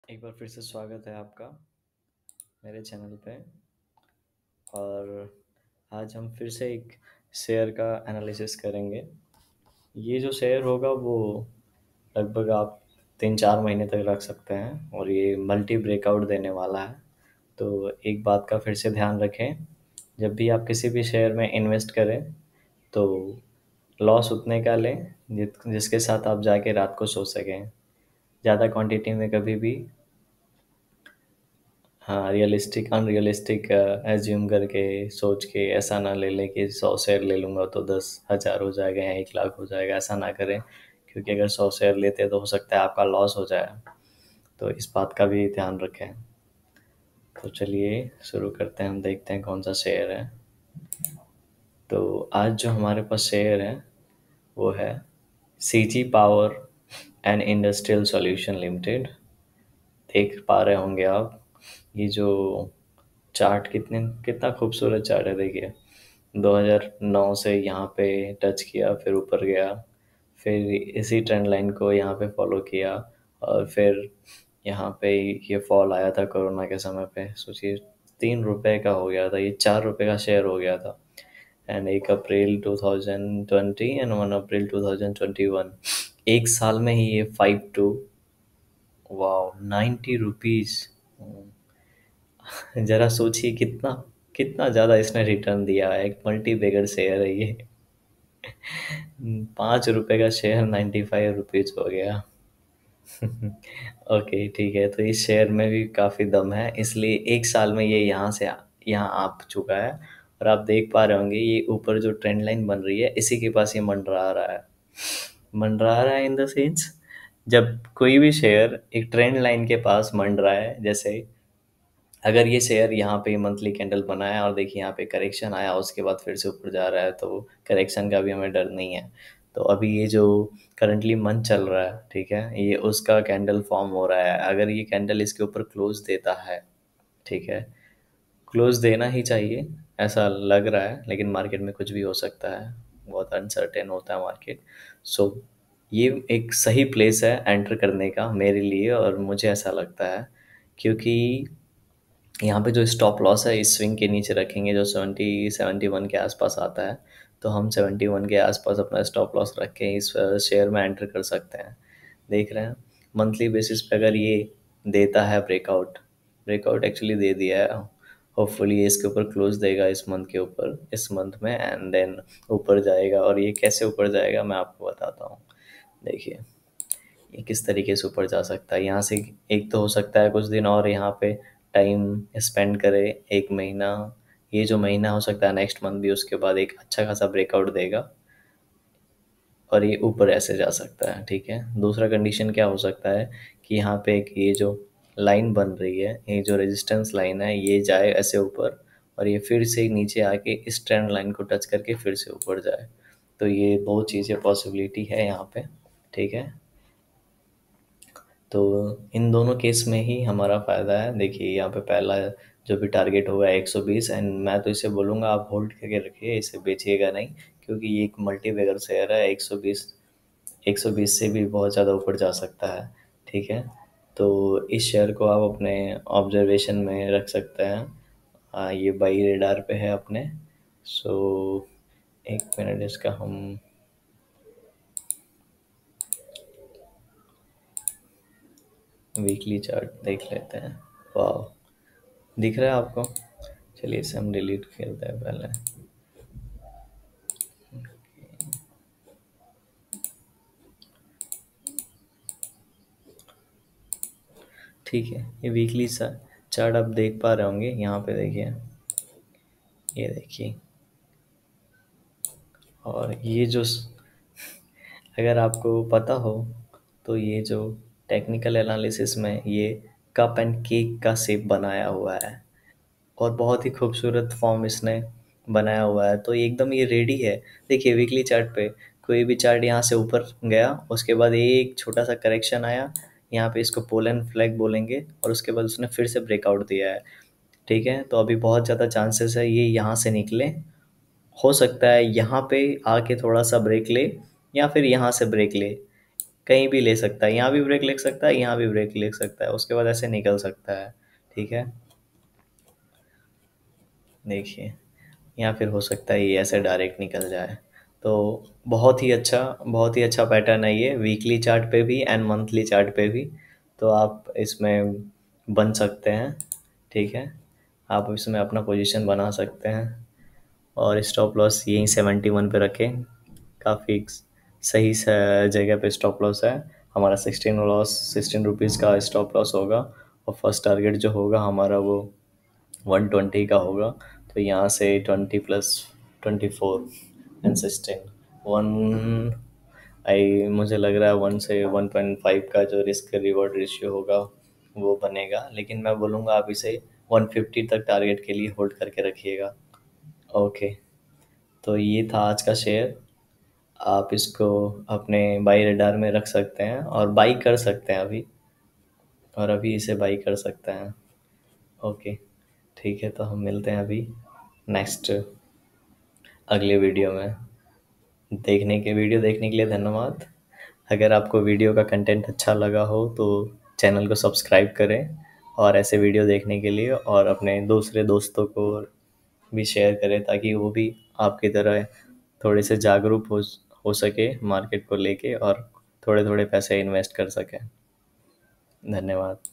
CG Power and Industrial Solutions limited share | Multi Year Breakout | Share to invest/Swing trade
CG Power and Industrial Solutions LTD share is giving multi year breakout, so hold for at leas 25 percent.CG power share is under buy radar. This can be a good pick to enter and hold for short term as well as long term gain.This share can give a big profit with small stop loss. Buy only if meet our terms. Please before taking any decision consult your advisor.